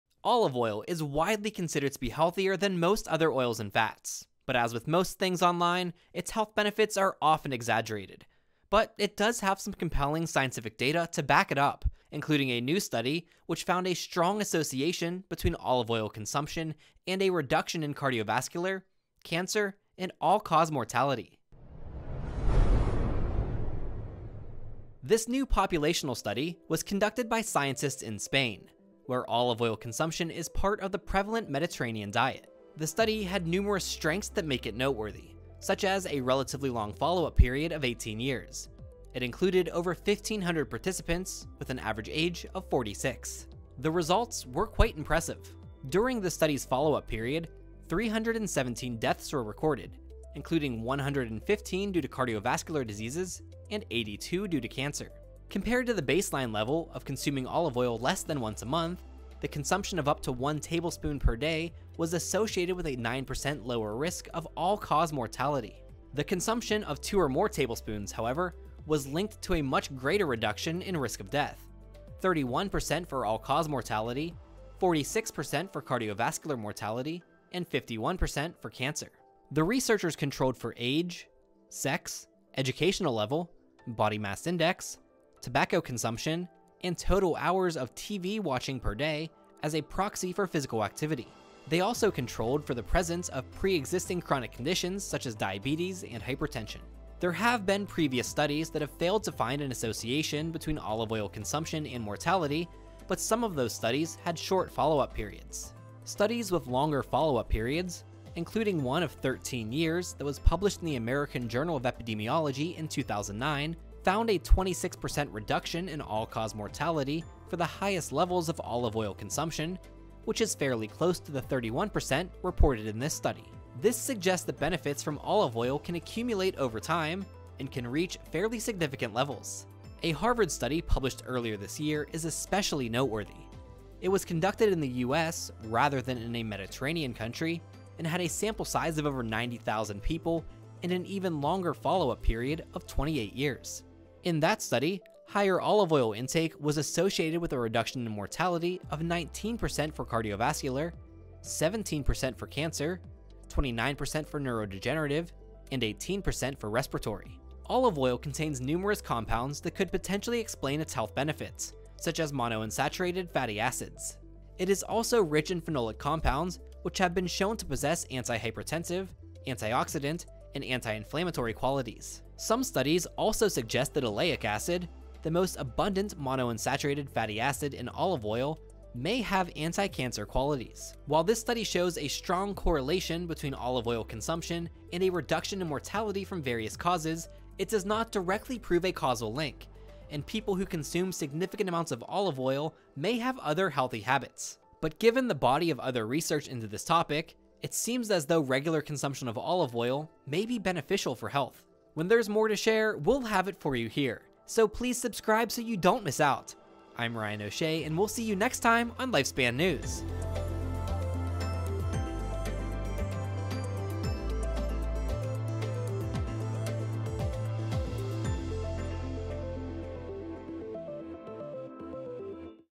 Do the Health Benefits of Olive Oil Reduce Risk of Death?
Olive oil is widely considered to be healthier than most other oils and fats, but as with many things online, its health benefits are often exaggerated. But some benefits are backed by compelling scientific evidence that is worth looking into, including a new study that found a strong association between olive oil consumption and lower risk of cardiovascular, cancer, and all-cause mortality.

This new populational study was conducted by scientists in Spain, where olive oil consumption is part of the prevalent Mediterranean diet. However, it has several strengths that make it noteworthy, including its relatively long follow-up period of 18 years. The study included over fifteen hundred adult participants, with an average age of 46.

The results were quite impressive. During the study’s follow-up period, 317 deaths were recorded, including 115 due to cardiovascular diseases and 82 due to cancer. Compared to the baseline level of consuming olive oil less than once per month, the consumption of up to one tablespoon per day was associated with a 9% lower risk of all-cause mortality. The consumption of two or more tablespoons, however, was linked to a much greater reduction in the risk of death: 31% for all-cause mortality, 46% for cardiovascular mortality, and a 51% for cancer.

The researchers  controlled for age, sex, educational level, body mass index, tobacco consumption, total hours of TV watching per day as a proxy for physical activity, and the presence of pre-existing chronic conditions such as diabetes and hypertension.

Some previous studies have indeed failed to detect an association between olive oil consumption and mortality, but some of those studies included short follow-up periods. Studies with longer follow up periods, including one with a follow-up period of 13 years that was published in the American journal of epidemiology in 2009, found a 26% reduction in all-cause mortality for the highest levels of olive oil consumption, which is fairly close to the 31% reported in this study. This suggests that benefits from olive oil consumption accumulate over time and can reach fairly significant levels.

A Harvard study published earlier this year is especially noteworthy. It has been conducted in the US rather than a Mediterranean country with a sample size of over 90,000 and an even longer follow-up period of up to 28 years. In that study, higher olive oil intake was associated with a 19% lower risk of cardiovascular mortality, a 17% lower risk of cancer mortality, a 29% lower risk of neurodegenerative mortality, and an 18% lower risk of respiratory mortality.

Olive oil contains numerous compounds that could potentially explain its health benefits, such as monounsaturated fatty acids. It is also rich in phenolic compounds which have been shown to possess antihypertensive, antioxidant, and anti-inflammatory qualities. Some studies also suggest that oleic acid, the most abundant monounsaturated fatty acid in olive oil, has anti-cancer qualities.

While this study shows a strong correlation between olive oil consumption and mortality from various causes, It does not directly prove a causal link, and people who consume significant amounts of olive oil might also have other healthy habits. Still, especially given the body of other research into this topic, it strongly suggests that regular consumption of olive oil is beneficial for health.

Adherence to the Mediterranean diet and risk of coronary heart disease in the Spanish EPIC Cohort Study =

Olive oil consumption and all-cause, cardiovascular and cancer mortality in an adult mediterranean population in Spain

Anti-tumor Effect of Oleic Acid in Hepatocellular Carcinoma Cell Lines via Autophagy Reduction

PRODUCTION CREDITS
Host, Producer & Editor - Ryan O’Shea
Writer - Arkadi Mazin